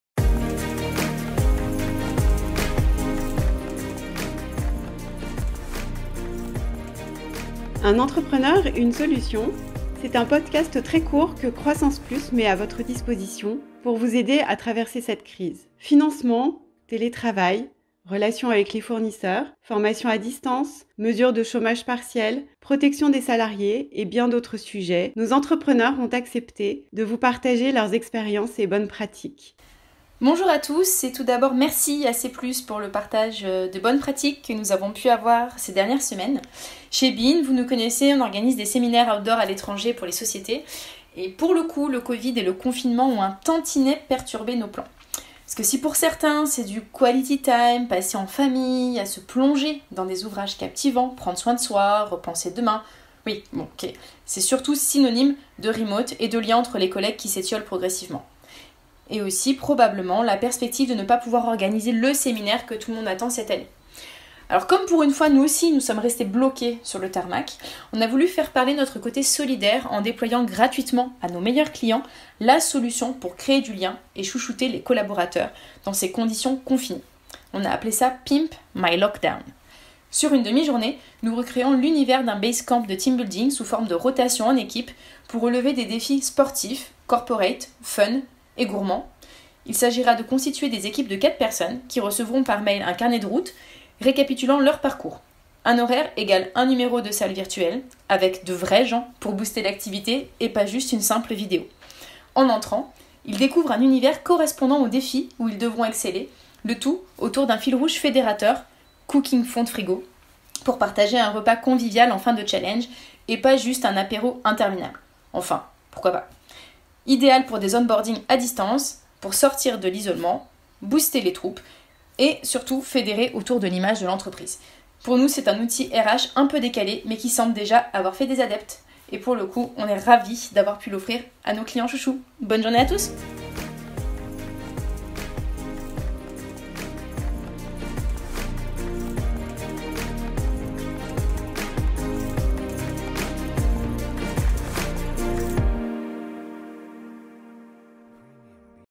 Un entrepreneur, une solution #22 - Juliette Rémy
Nos entrepreneurs ont accepté de partager leur expérience et leurs bonnes pratiques pour vous aider à traverser la crise !
Comment Juliette Rémy, CEO de Bee In, a adapté son offre pendant cette période ? Ecoutez son podcast !
Disponible sur SoundCloud, Spotify et iTunes.

A propos :
CroissancePlus est le 1er réseau d'entrepreneurs de croissance engagés pour libérer l'économie et favoriser la croissance des entreprises.